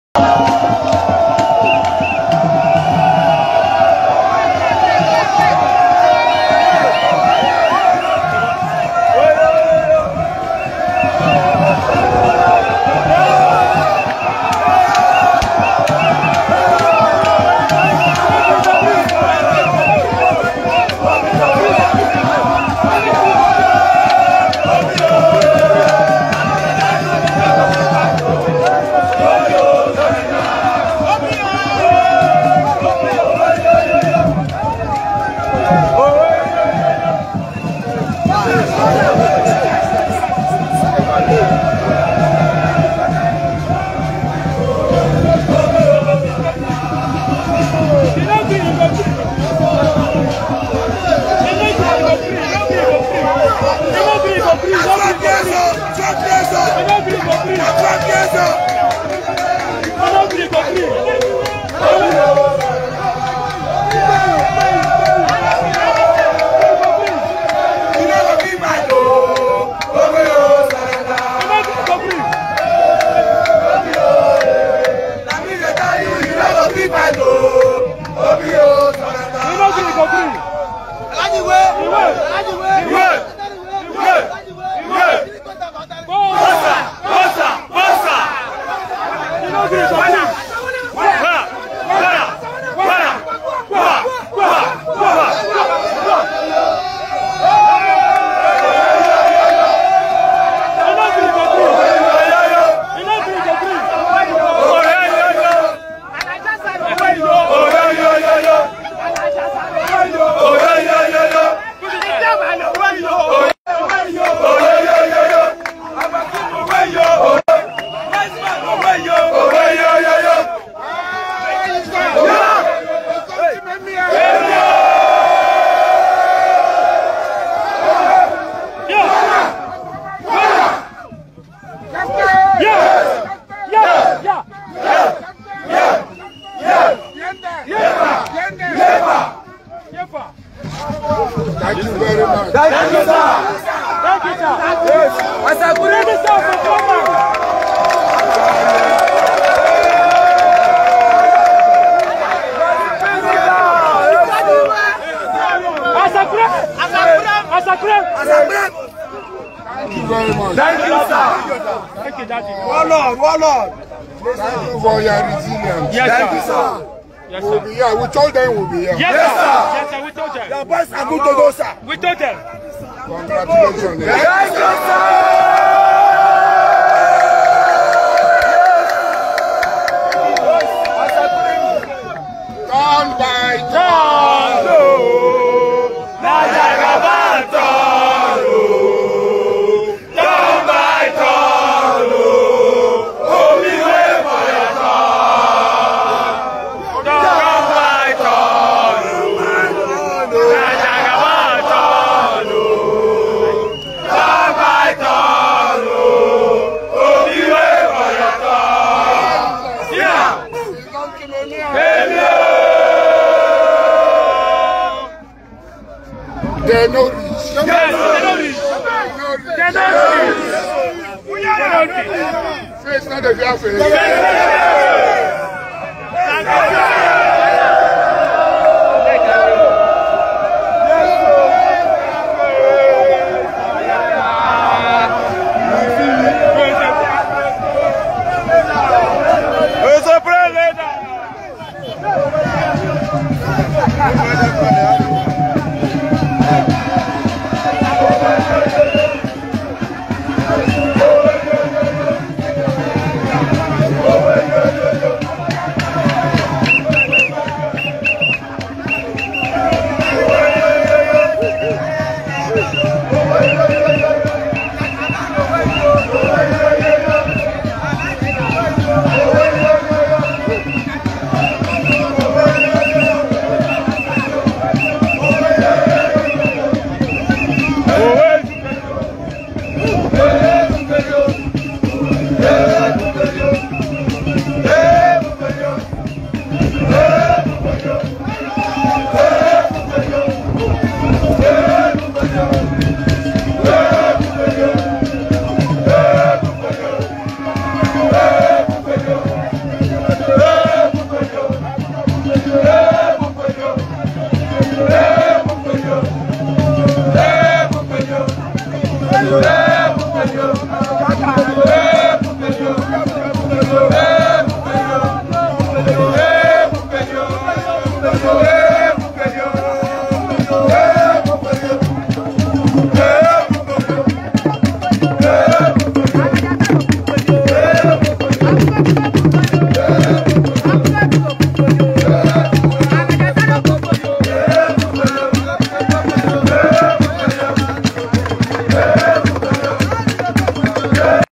Asari Dokubo & his boys have arrived Abuja, according to him, to ensure that Tinubu’s mandate stays
They also ask Peter Obi of Labour Party to wait for his turn